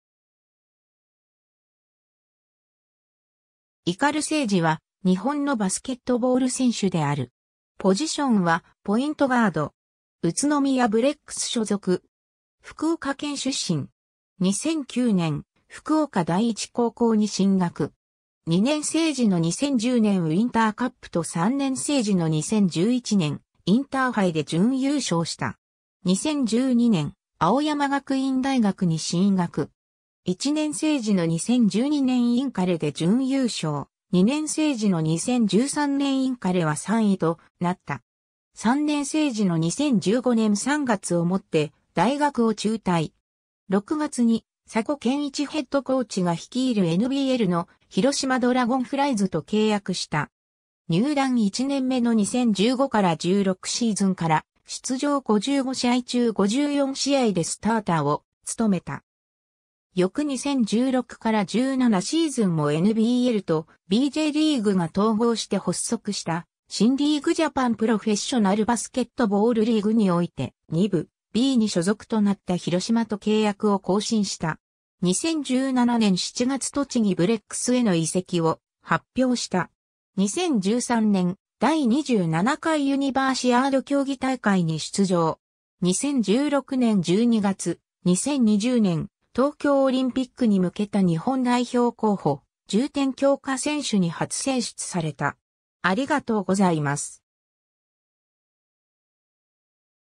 鵤誠司
鵤 誠司（いかるが せいじ、1994年1月8日 - ）は、日本のバスケットボール選手である。
宇都宮ブレックス所属。
 福岡県出身。
2009年、福岡第一高校に進学。
2年生時の2010年ウインターカップと3年生時の2011年インターハイで準優勝した。
 2012年、青山学院大学に進学。
1年生時の2012年インカレで準優勝、2年生時の2013年インカレは3位となった。
 3年生時の2015年3月をもって大学を中退。
6月に佐古賢一ヘッドコーチが率いるNBLの広島ドラゴンフライズと契約した。
入団1年目の2015-16シーズンから出場55試合中54試合でスターターを務めた。
 翌2016-17シーズンもNBLとbjリーグが統合して発足した新リーグ・ジャパン・プロフェッショナル・バスケットボールリーグ（Bリーグ）において、2部（B2)所属となった広島と契約を更新した。 2017年7月栃木ブレックスへの移籍を発表した。 2013年、第27回ユニバーシアード競技大会に出場。 2016年12月、2020年東京オリンピックに向けた日本代表候補重点強化選手に初選出された。